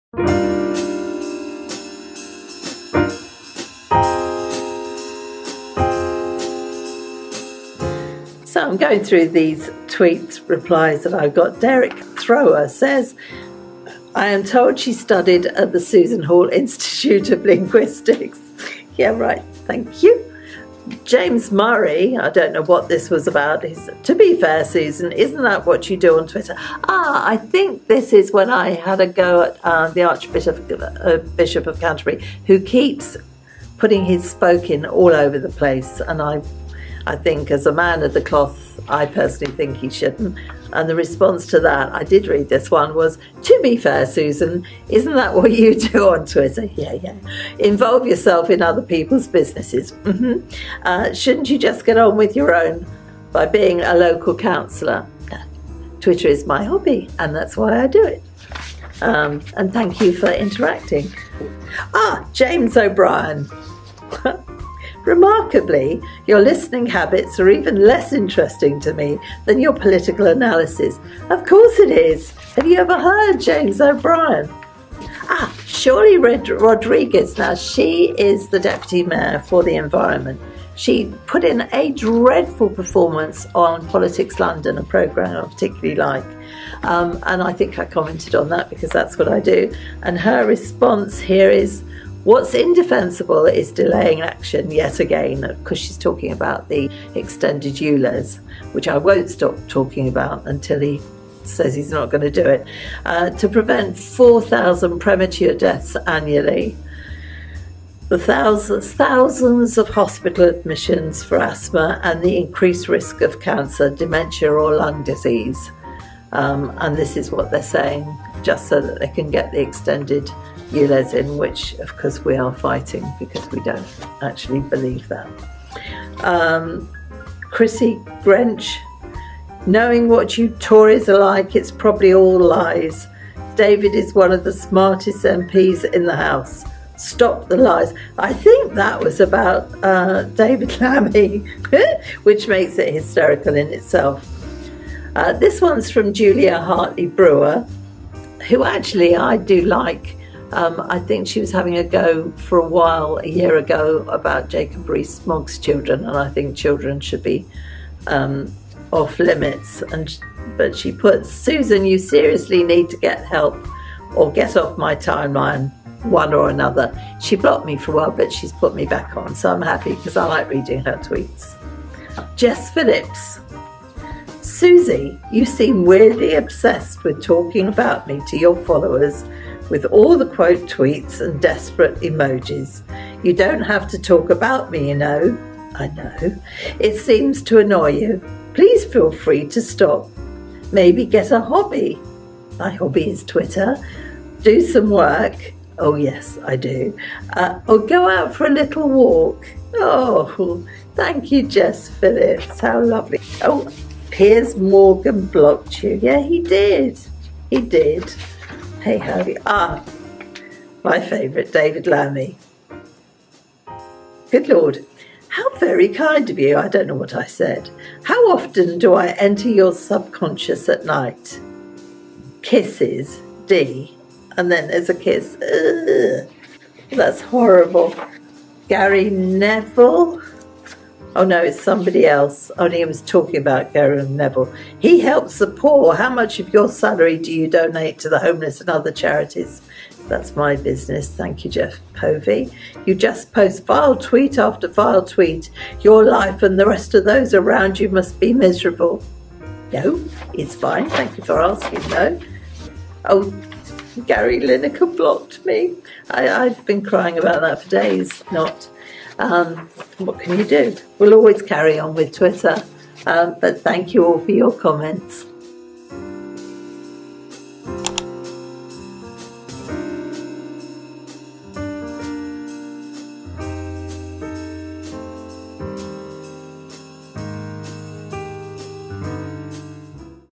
Susan Hall REACTS to mean tweets! 😂
Susan Hall AM confronts the haters...

0:00 Intro
1:01 James O'Brien
1:16 Sadiq Khan's deputy
2:08 David Lammy fan
2:25 Julia Hartley-Brewer
2:51 Jess Phillips
3:30 David Lammy
3:48 Gary Neville fan
4:20 Outro